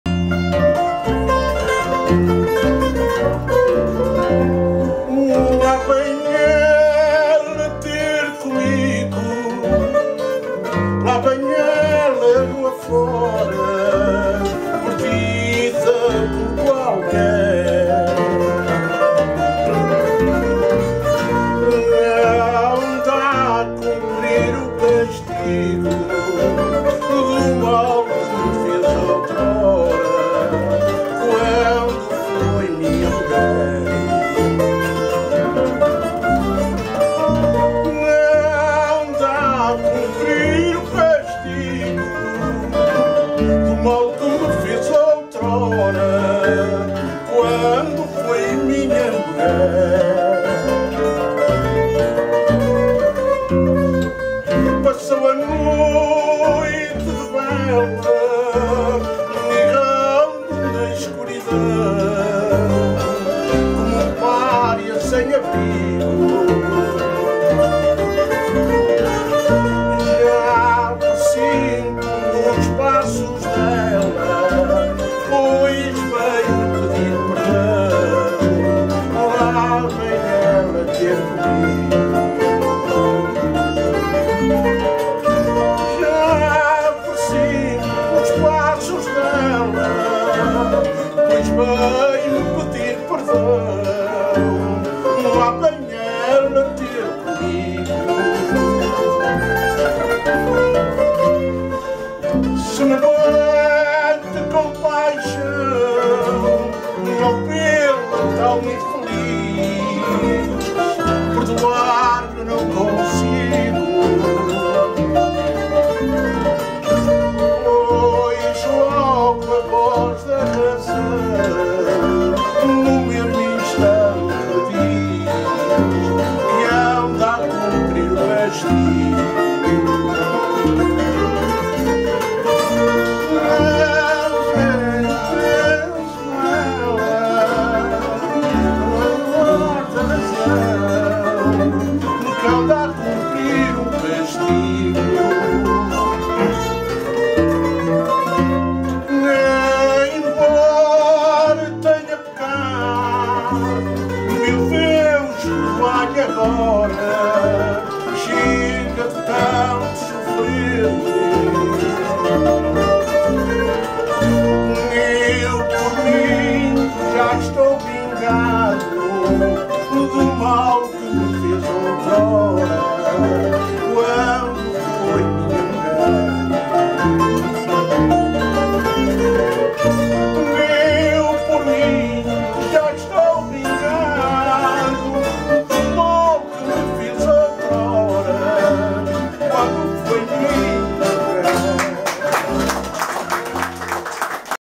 MANUEL DOS SANTOS FADO, NA CASA DO FADISTA EM FÂNZERES GONDOMAR
O FADISTA , MANUEL DOS SANTOS , PRESENTE NA CASA DO FADISTA NA RUA DE CIMO DA COSTA Nº 142, EM FÂNZERES GONDOMAR, PERTO DO ALTO DA SERRA SE SÃO PEDRO DA COVA , CASA DE ENSAIOS AOS FADISTAS DO NORTE , PARA BEM DA CULTURA , PARA ENSAIAREM OS SEUS FADOS , E TIRAREM OS SEUS TONS COM JORGEFERREIRAFADO ,  EM VIDEO AUTORIZADO E SEM FINS LUCRATIVOS , EM TUDO ISTO É FADO